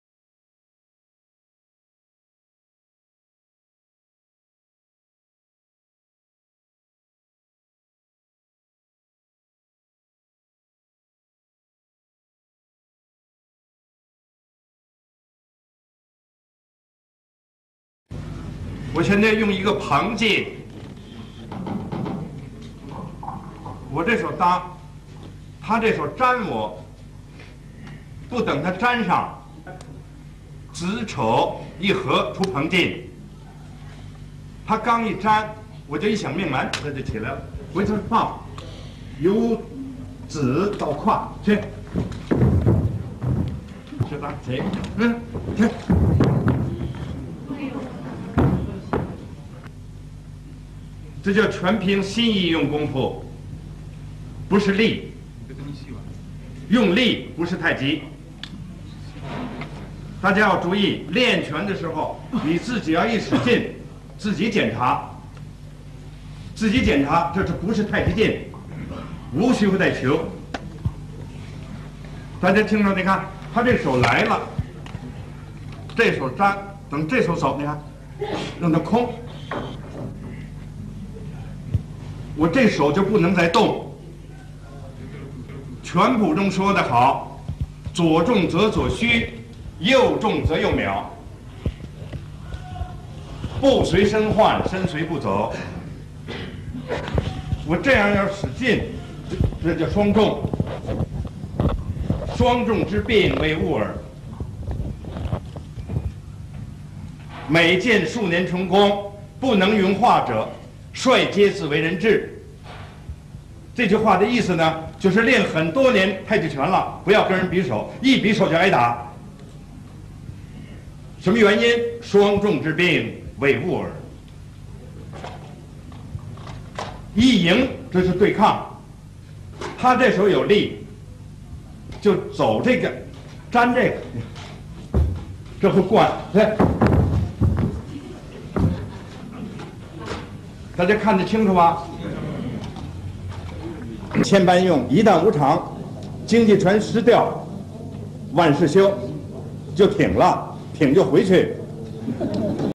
Wang Peisheng Tai Chi Pushing Hands at its advanced level
Wang Peisheng delivers here his secret trick to overcome any opponent in Taichi  Pushing Hands. He explains movements and corresponding Chinese acu-points to focus during movements in addition to the philosophical idea beneath it. This is useful not only in Wu style but merely in any Tai chi style.